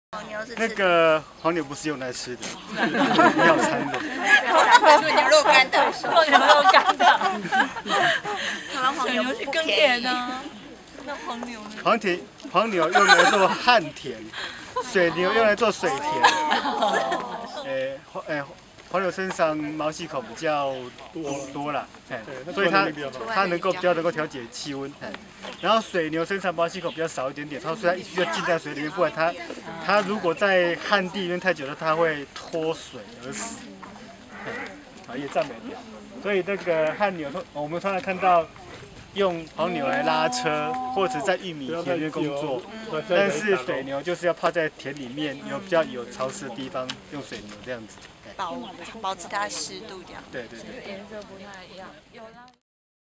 黃牛與水牛的差別在哪裡 ?
李坤昇建築師於柳營「老牛的家」，充當一日導遊向民眾介紹「黃牛」與「水牛」不同的地方與特性。